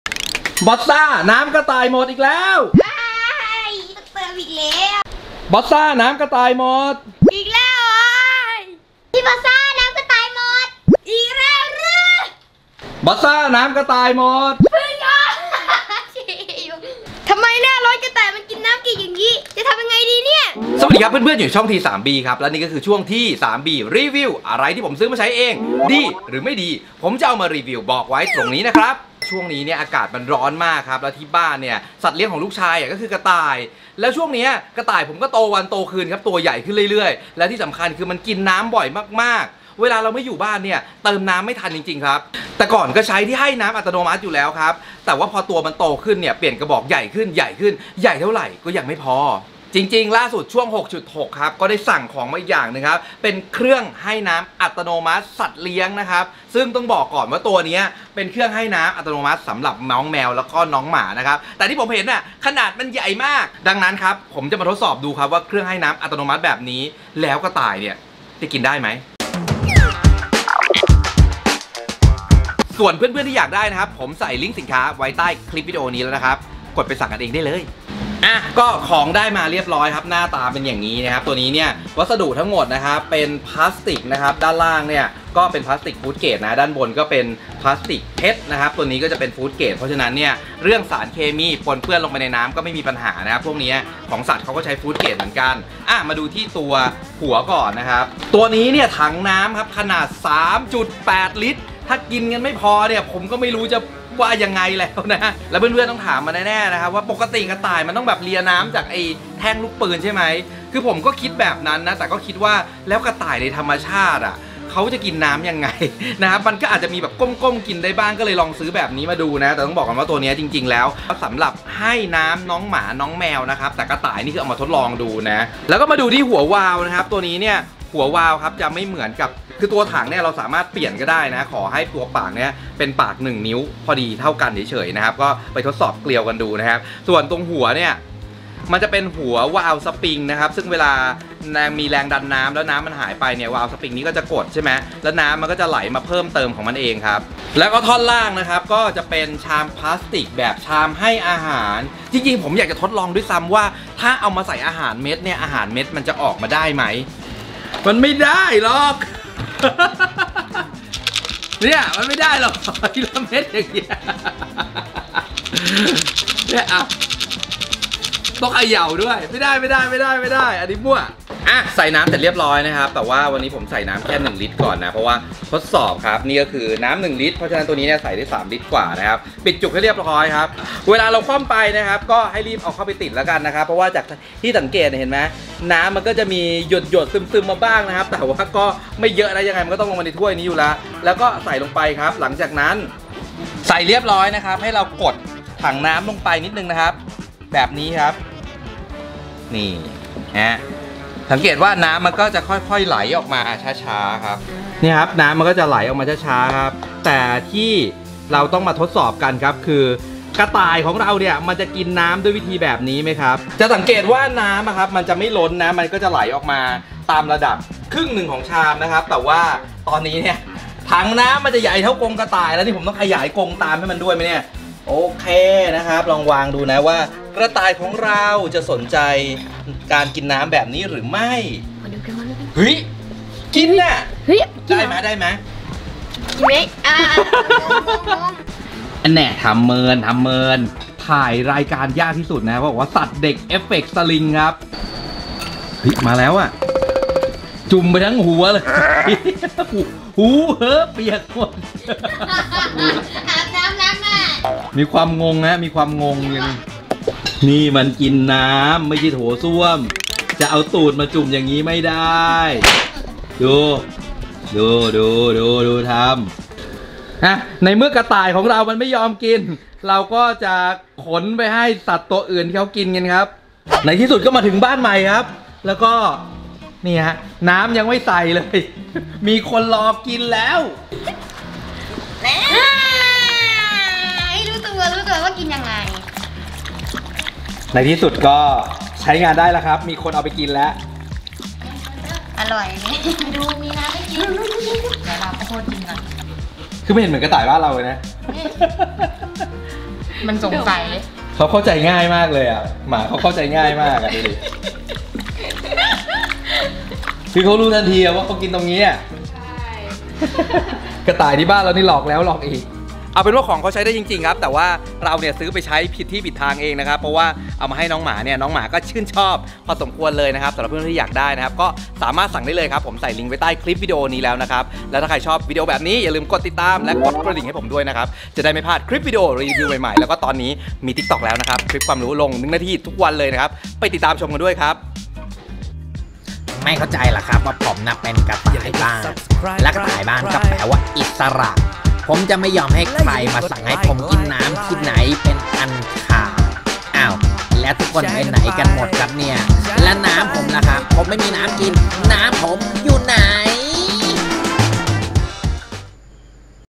รีวิว ที่ให้น้ำอัตโนมัติ สัตว์เลี้ยง Shopee ร้านCetdoghappyshop l T3B
ช่วงนี้อากาศร้อนมากเติมน้ำให้ทั้งวันก็ไม่พอ เลยหาเครื่องทุ่นแรงมาครับ สำหรับเพื่อนๆที่มีน้องหมา น้องแมว OK เลยผมแนะนำครับ แต่กระต่ายบ้านผมไม่ถนัดจริงๆ